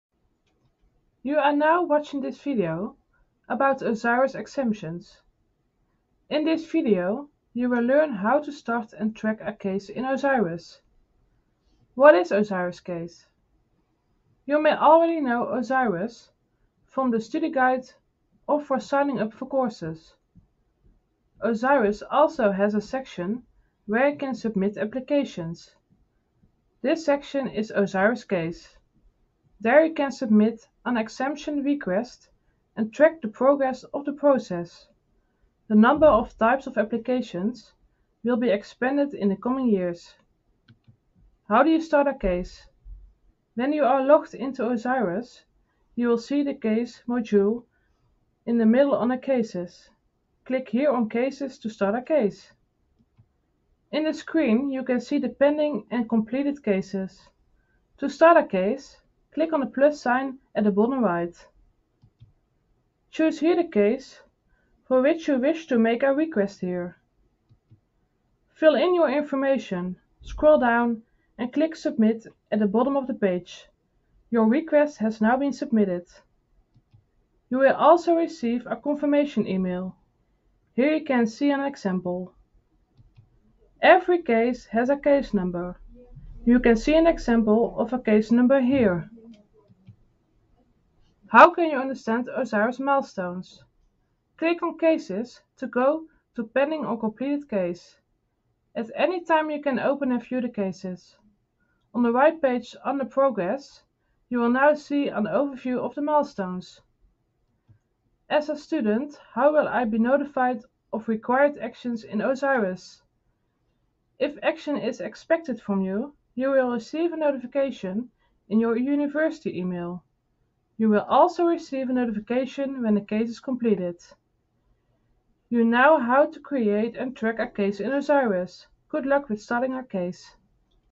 Knowledge clip for students - Tilburg University
In this video you will learn how to start and track a case in Osiris - Tilburg University